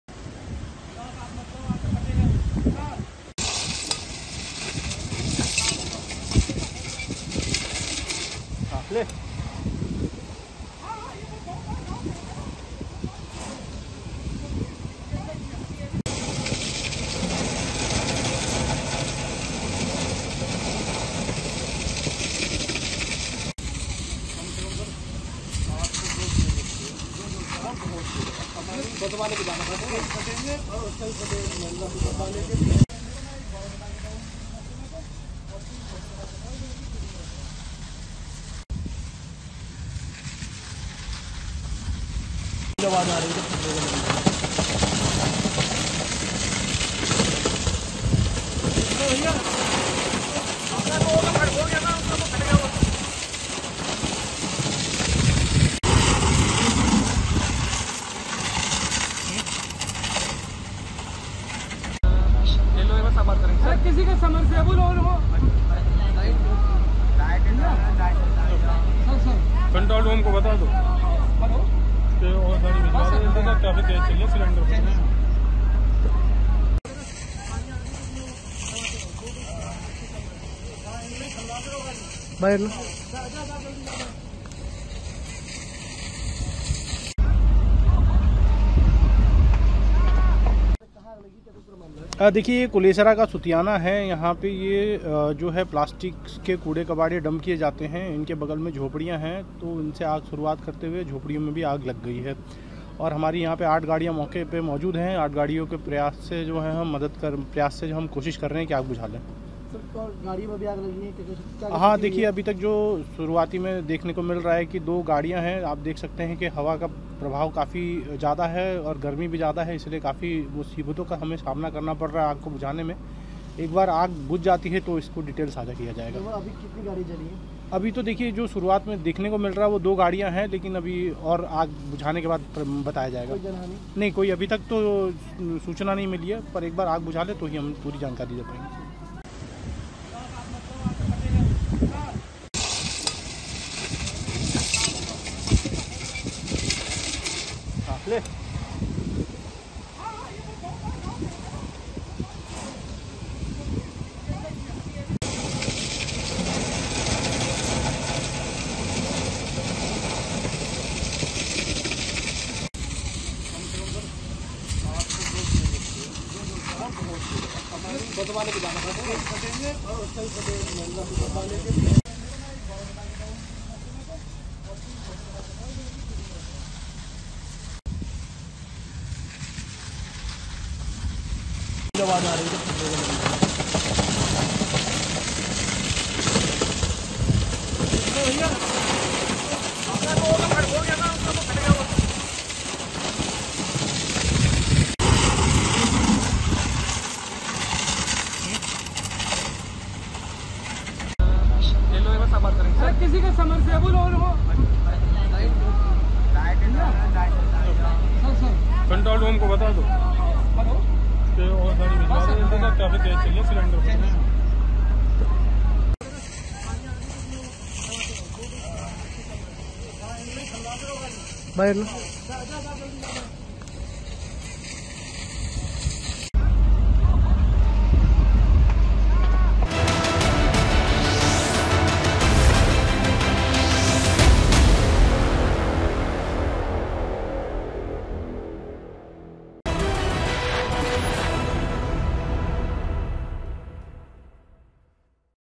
Greater Noida के Kulesra में आग ने मचाया हाहाकार, इलाके में छाया काला धुआं | Breaking News | Fire
Greater Noida Fire: ग्रेटर नोएडा के कुलेसरा में 5 मई को भीषण आग लग गई. आग लगने से पूरे इलाके में हड़कंप मच गया है और लोगों में अफरा-तफरी का माहौल है. फिलहाल आग को बुझाने में दमकल की गाड़ियां जुटी हुई हैं. आग लगने से इलाके में काले धुए का गुबार छा चुका है, देखें वीडियो.